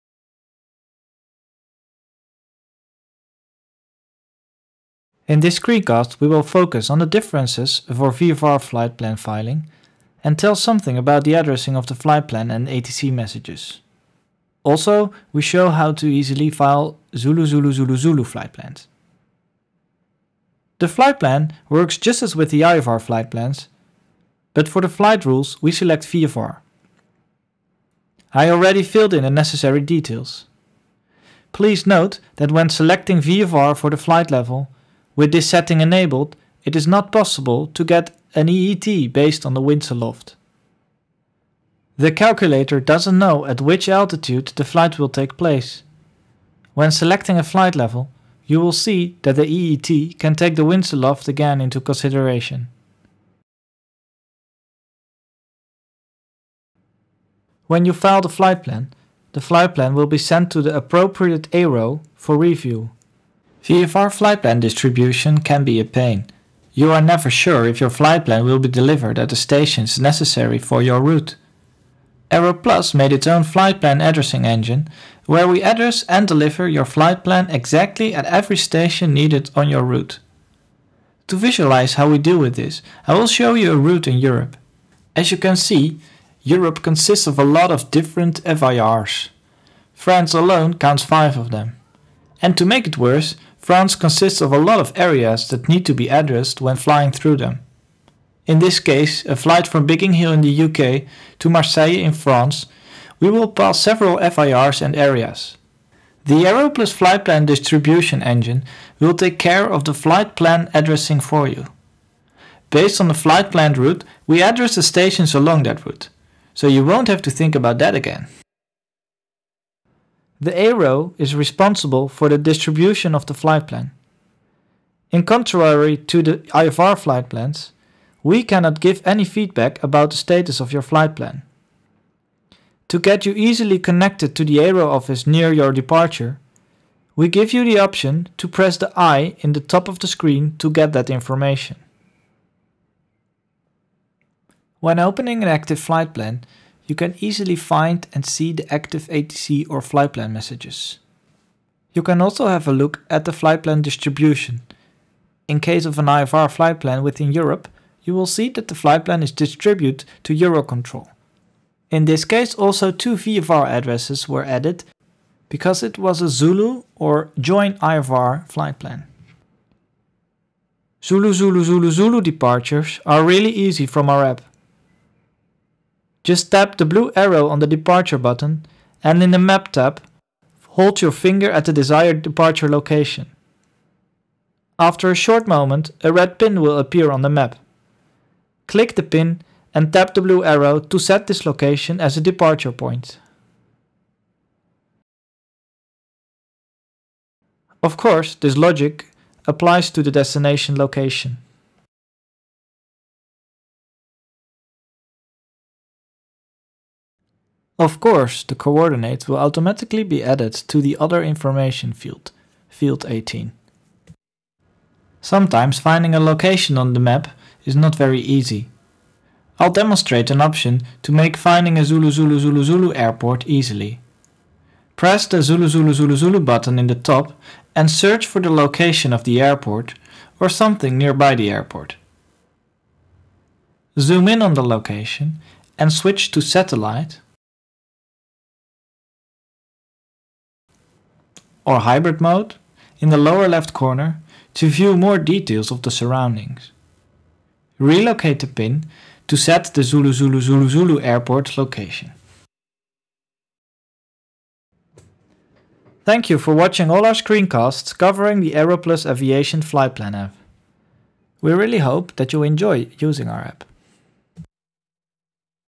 AeroPlus Flightplan App Tips on ZZZZ, VFR, notifications and flightplan distribution
Tips for making VFR flight plans, ZZZZ departures and arrivals and the different notifications handled in our app. Also some explanation about the AeroPlus Flight Plan distribution engine.